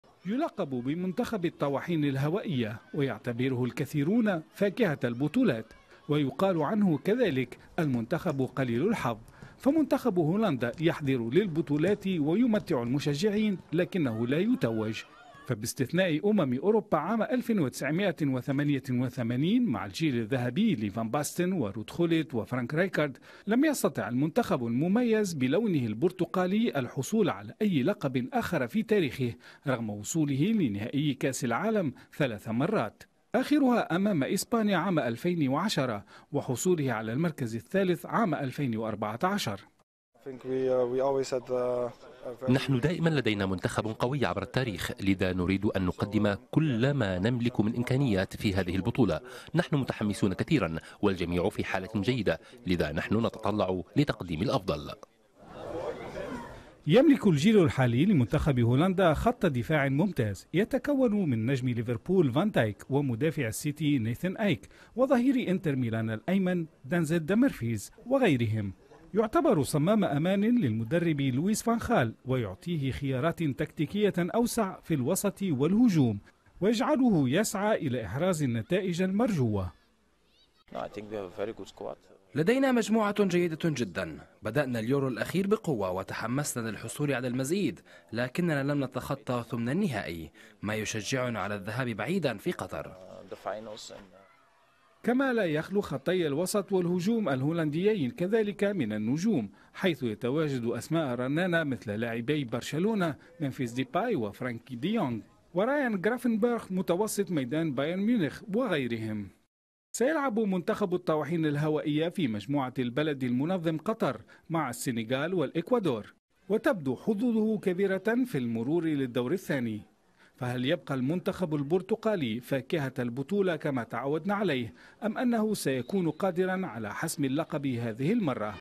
المنتخب الهولندي في مونديال قطر .. الملك غير المتوج يبحث عن لقبه الأول في كأس العالم
يعد منتخب هولندا أحد أكثر المنتخبات  جمالية في الأداء الكروي في تاريخ كأس العالم إلا أنه فشل في التتويج باللقب رغم حلوله وصيفا في ثلاث نسخ، فهل يحقق أول ألقابه في مونديال قطر.
تقرير: هيثم الغربي

ابعوا أبرز الأخبار، وآخر مستجدات الأحداث العربية والعالمية، عبر البث المباشر للتلفزيون العربي على يوتيوب